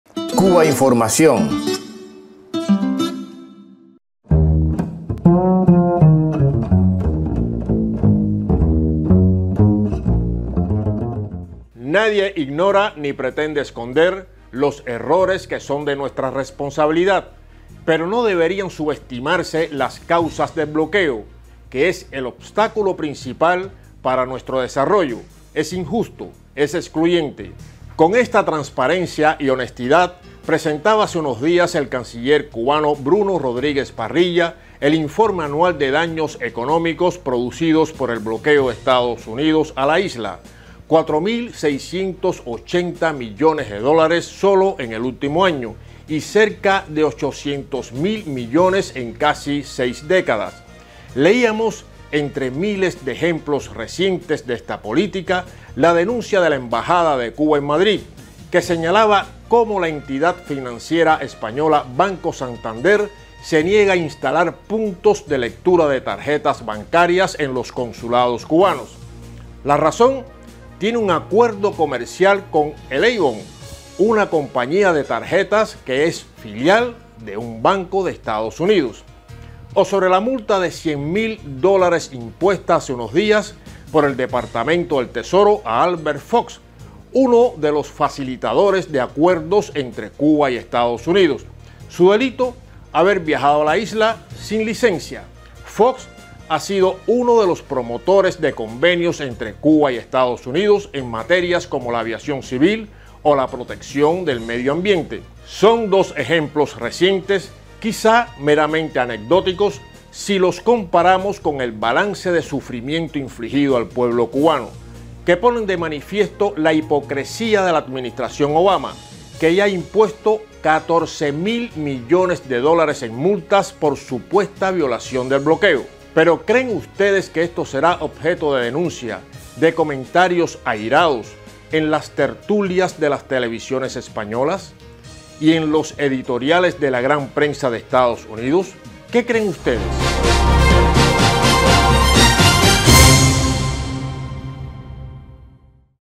14.000 millones en multas, 4.800 millones al año en daños económicos: ¿Cuba ya no sufría el bloqueo?
Cubainformación TV.- "Nadie ignora ni pretende esconder los errores que son de nuestra responsabilidad, pero no deberían subestimarse las causas del bloqueo, que es el obstáculo principal para nuestro desarrollo, es injusto, es excluyente". Edición: Ana Gil. Fuente